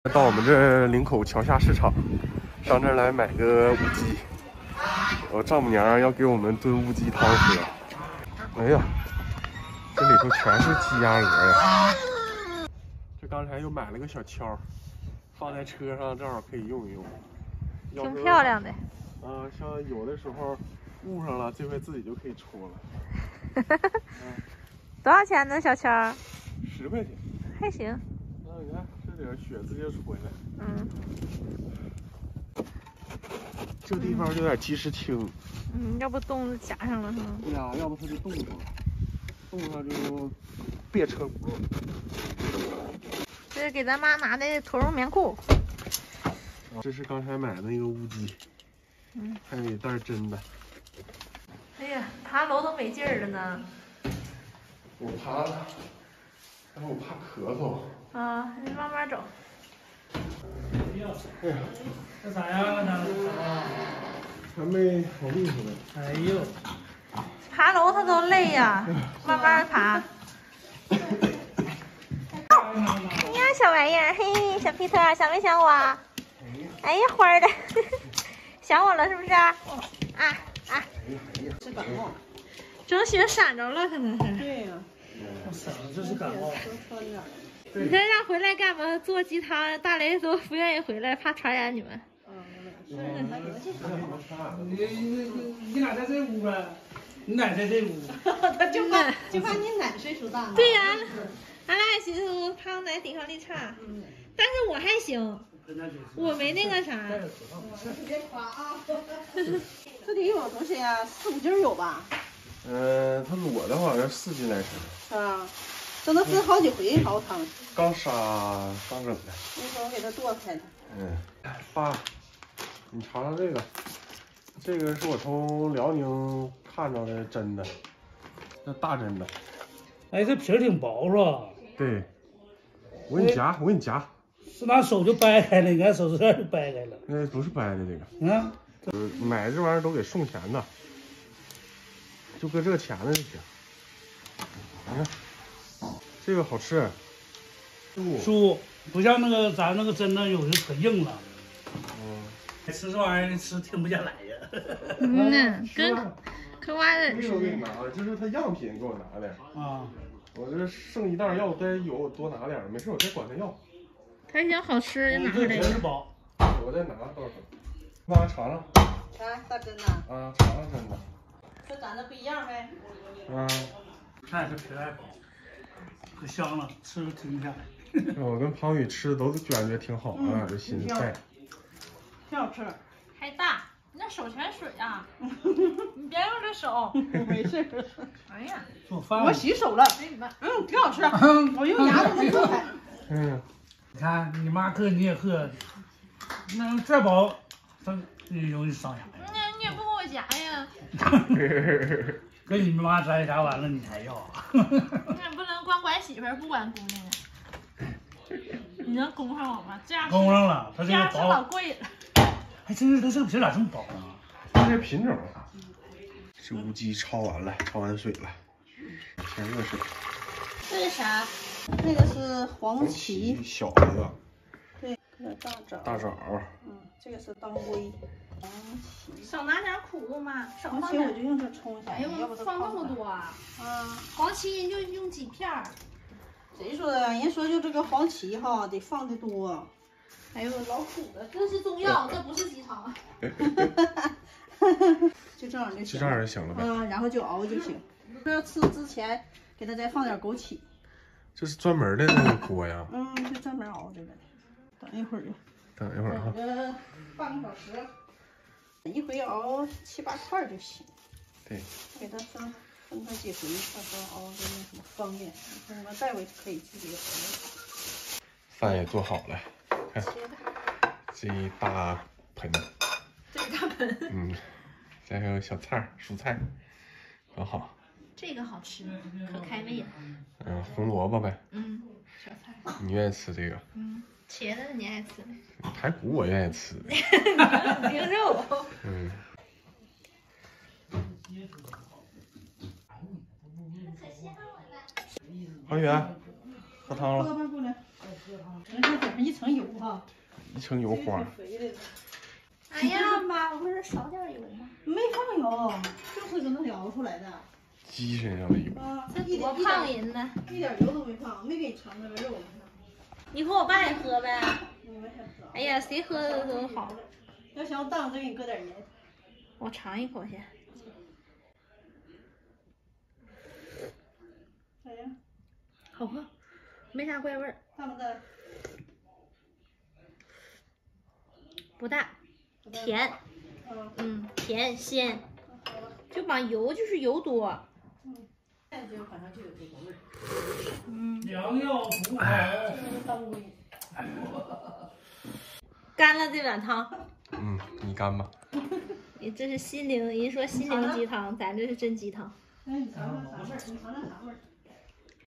大雷到市场买只大乌鸡，让丈母娘帮忙炖乌鸡气血汤，给媳妇补一补
大雷到市场买只大乌鸡，让丈母娘帮忙炖乌鸡气血汤，给媳妇补一补#林区大雷